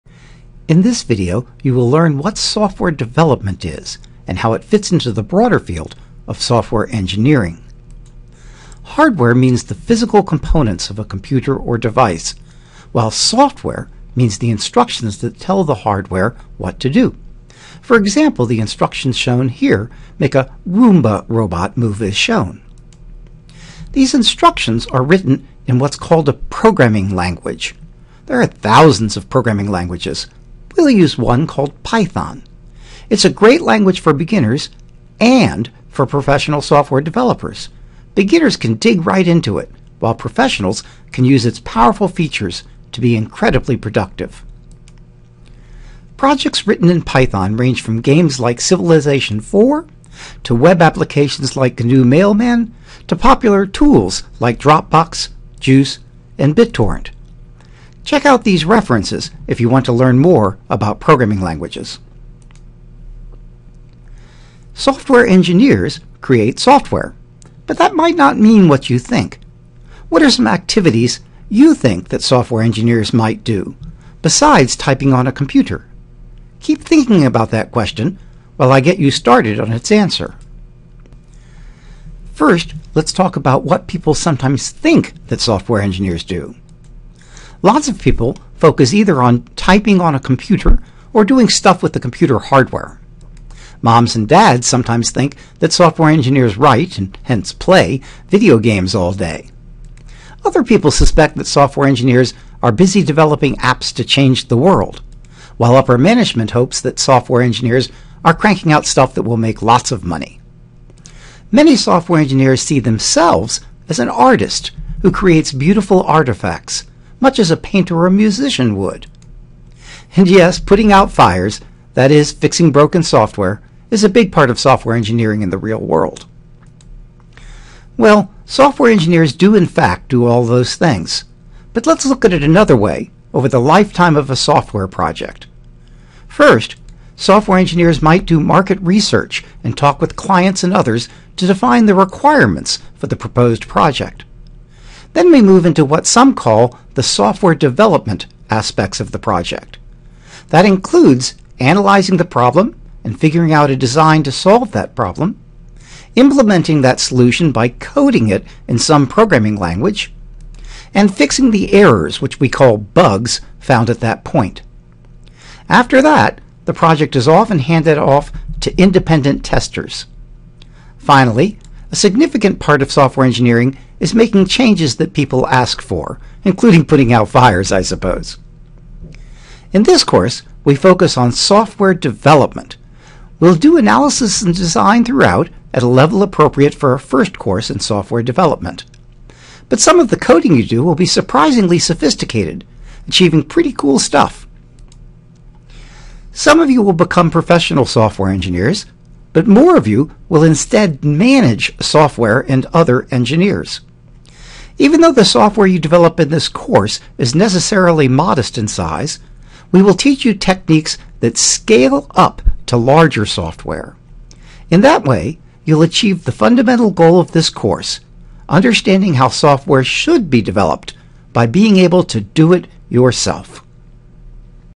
CSSE120 - 01 Intro to Python - 02 What Is Software Engineering?
This video is part of a course series Introducing Software Development using Python at Rose-Hulman.